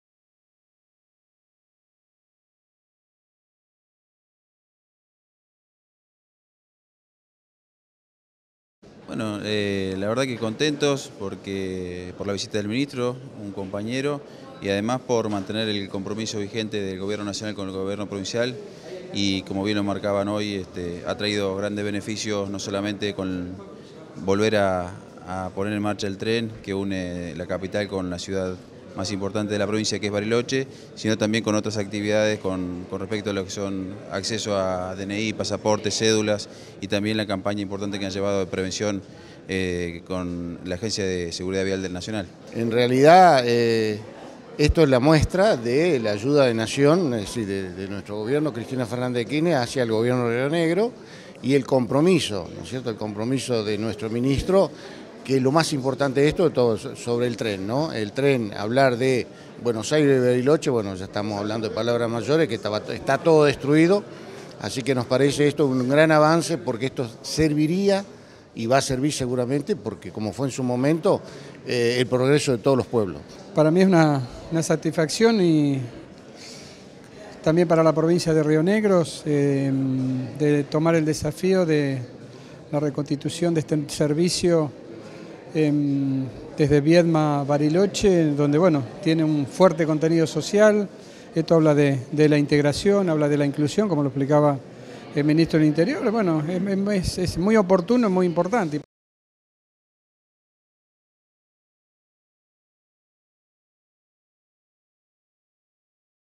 Intendentes, Dantas, Montanaro y Curetti: opinión sobre visita de Randazzo a Viedma
Los intendentes de Campo Grande, Pedro Dantas, Cervantes Gilberto Montanaro y de Patagones, Ricardo Curetti, fueron parte de la comitiva de autoridades que recibieron hoy al ministro del Interior y Transporte, Florencio Randazzo, quien arribó acompañado por el senador Nacional por Río Negro, Miguel Pichetto.
Durante el recorrido los intendentes manifestaron su opinión sobre la presencia del ministro y de los funcionarios nacionales que visitaron la ciudad de Viedma.